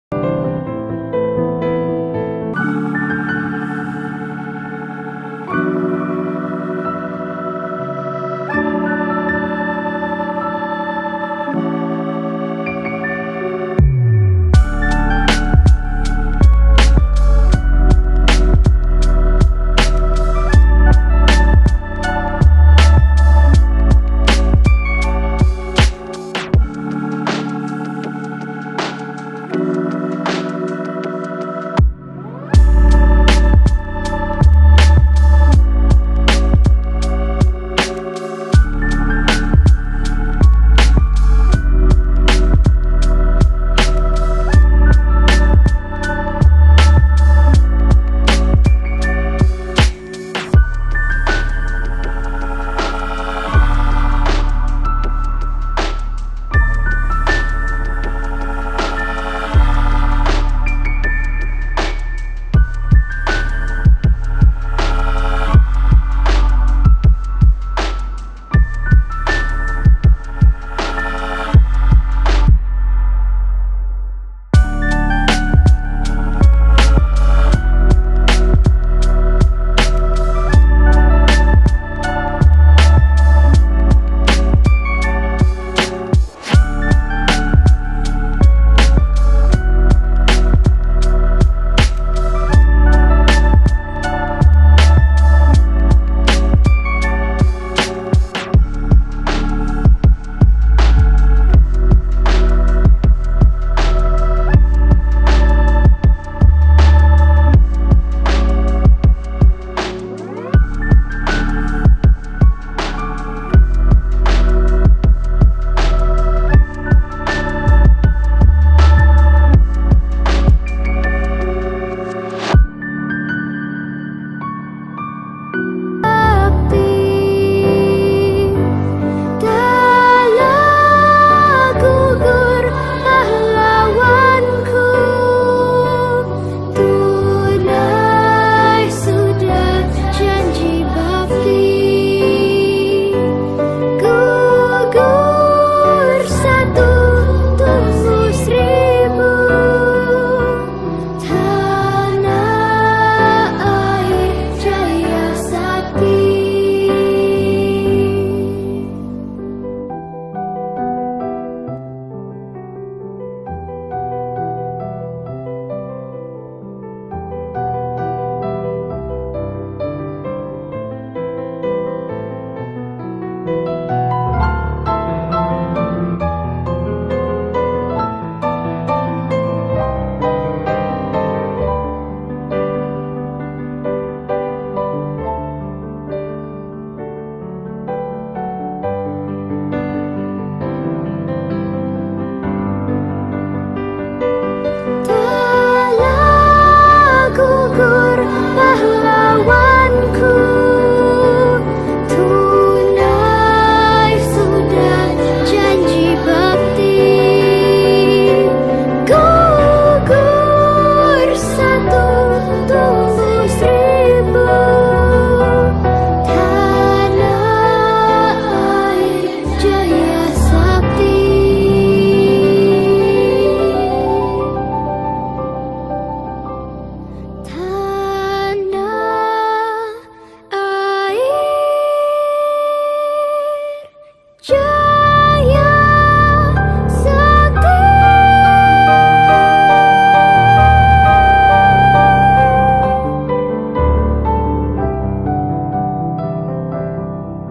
Turut Berduka Cita Atas Meninggalnya Letjen TNI Herman Asaribab | WAKASAD putra PAPUA
Beliau Meninggal Karna Sakit
Semoga Amal Ibadahnya Di Terima ALLAH SWT
AMIN